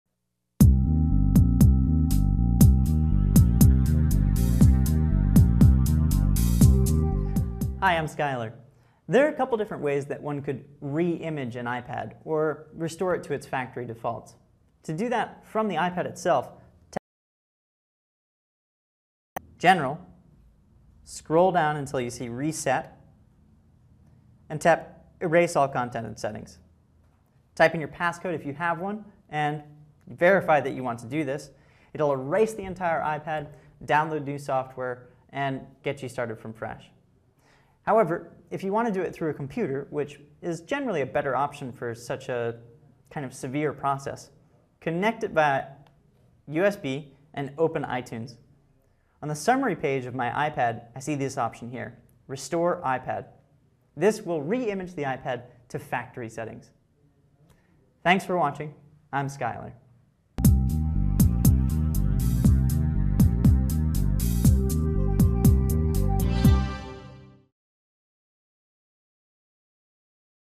How to Reimage an iPad
How to Reimage an iPad. Part of the series: Tech Yeah!. Reimaging on an iPad is something that requires you to follow a few very specific steps that begin within the "Settings" application. Reimage an iPad with help from an experienced electronics professional in this free video clip.